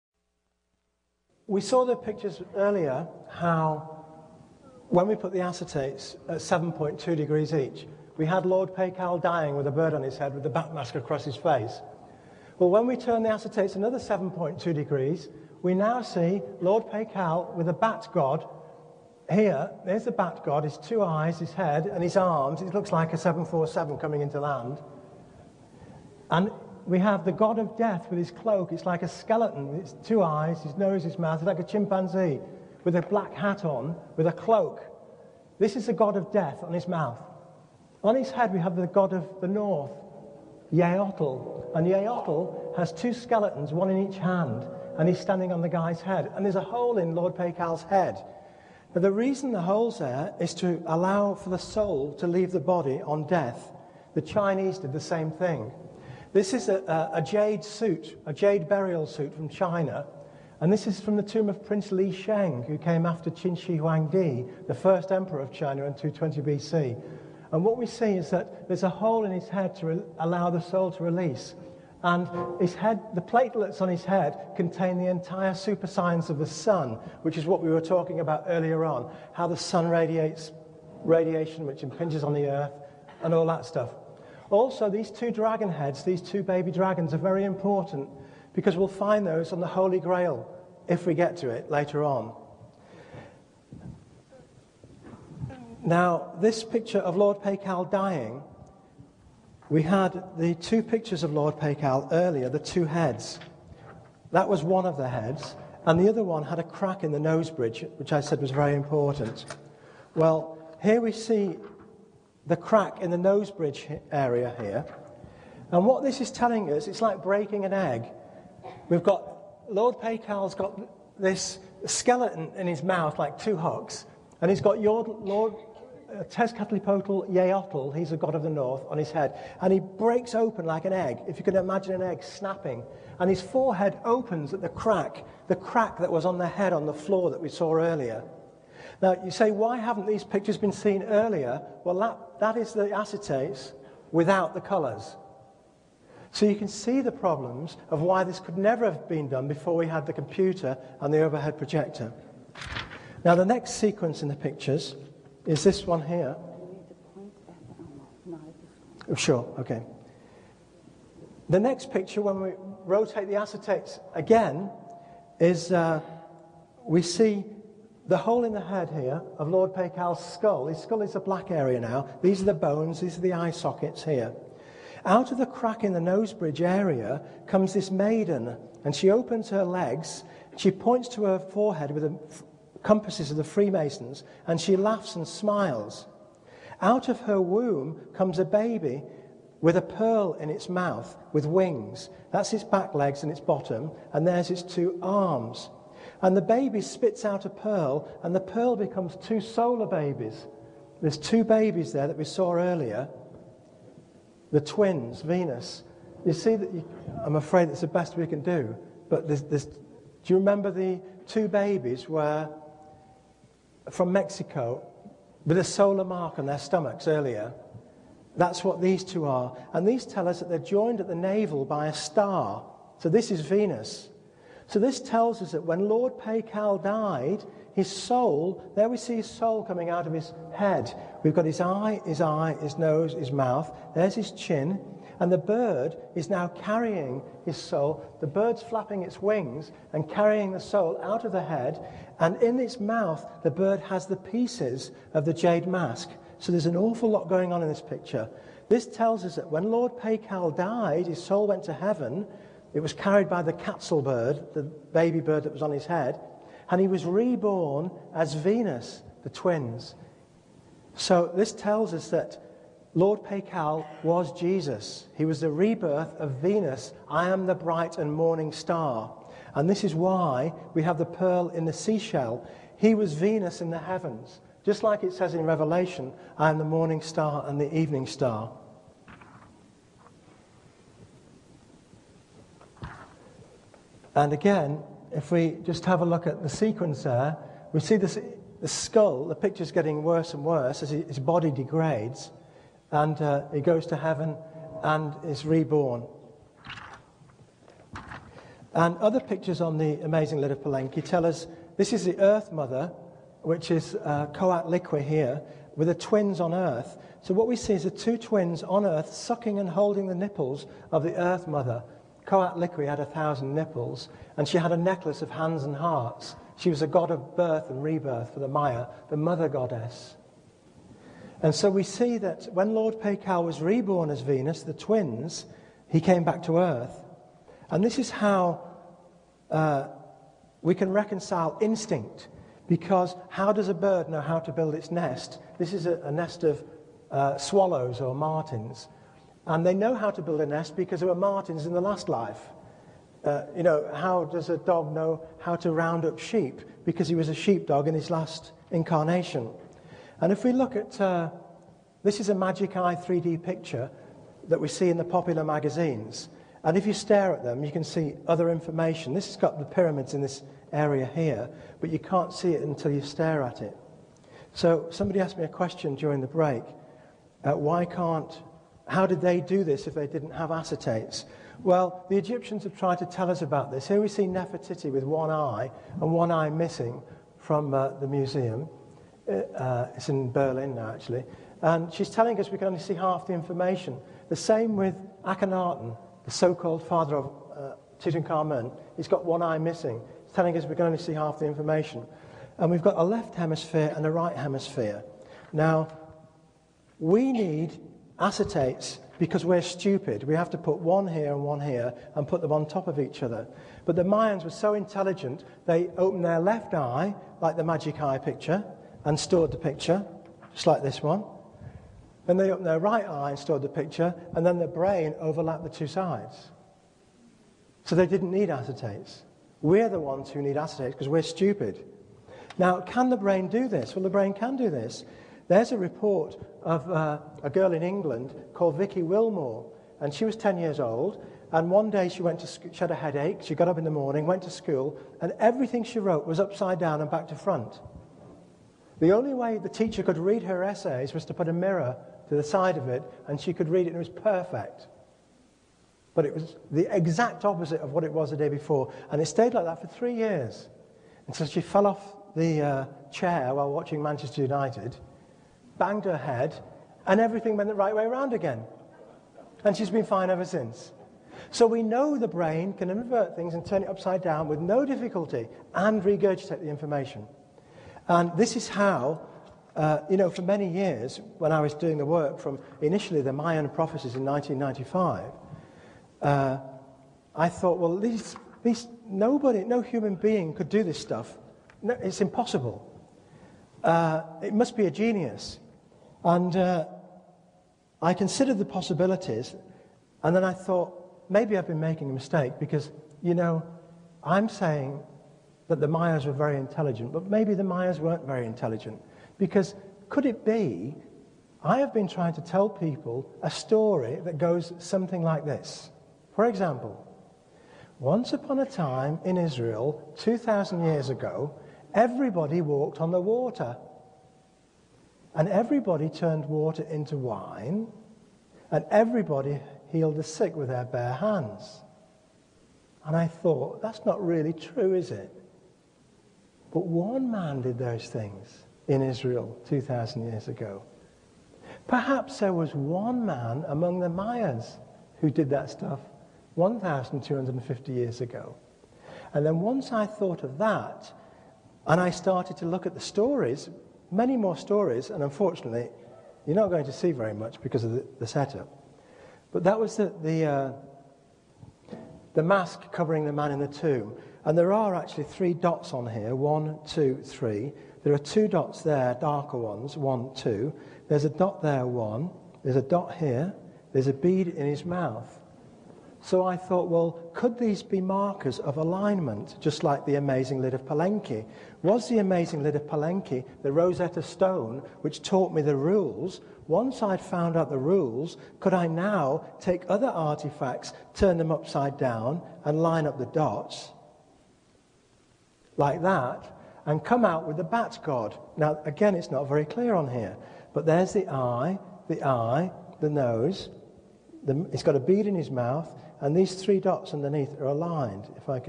Maurice Cotterell - Secrets of the Super Gods 11/19
In 1989 engineer and scientist Maurice Cotterell found a way of calculating the duration of long-term magnetic reversals on the Sun. Using this knowledge he was able to break the codes of ancient sun-worshipping civilizations, first the Mayas of Central America, those of Tutankhamen, of Egypt, and Viracocha of South America, before cracking the codes of the Terracotta Warriors of China.